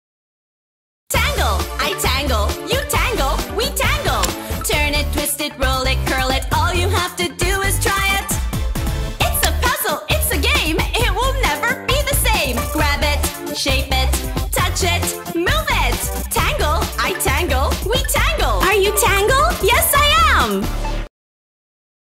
Tangle - The Original Fidget Toy at B&M Stores
Twist, shape, fidget!

The original fidget toy, a pioneer in therapeutic brain tools for children. Sensory and motor stimulation provided by Tangle can help stimulate and maintain attention, even improve math and reading comprehension.

Classic Tangle features bright colors and an ergonomic design for keeping fidgeters of all ages engaged!

For ages 3+ years. Adult supervision required at all times.

Browse more toys and games at B&M stores, both in-store